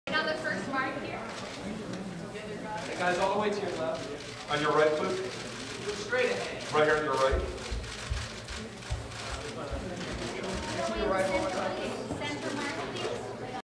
Quvenzhane Wallis, Paul Hoen at 65th Annual Directors Gui...
Quvenzhane Wallis, Paul Hoen at 65th Annual Directors Guild Of America Awards - Press Room 2/2/2013 in Hollywood, CA.